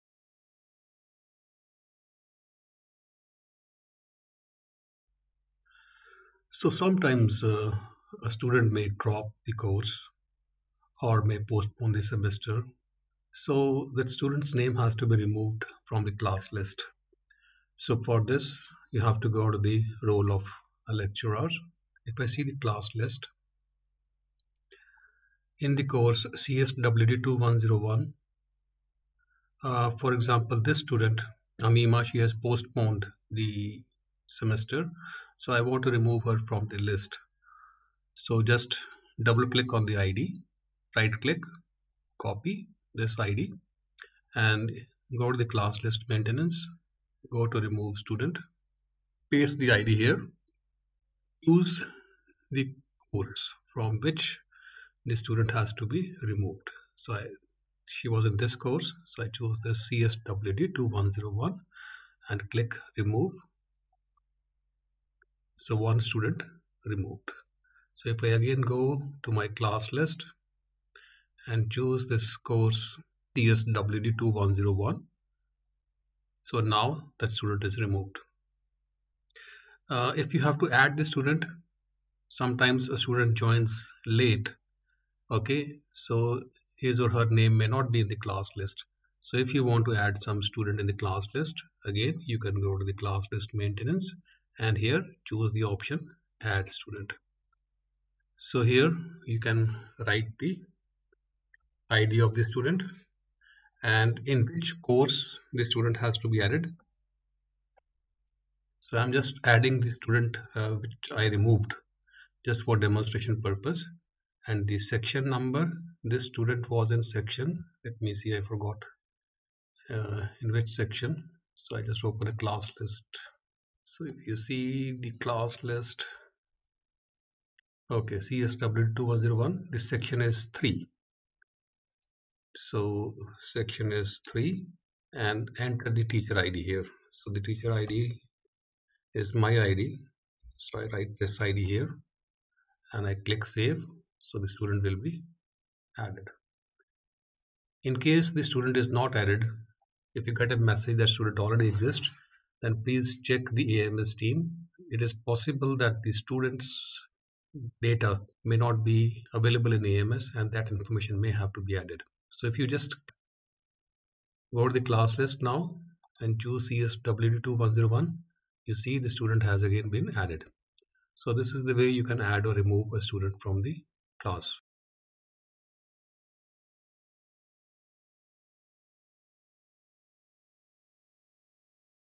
06 Adding Removing Students in Class List
The video explains the process of adding/removing students from the class list.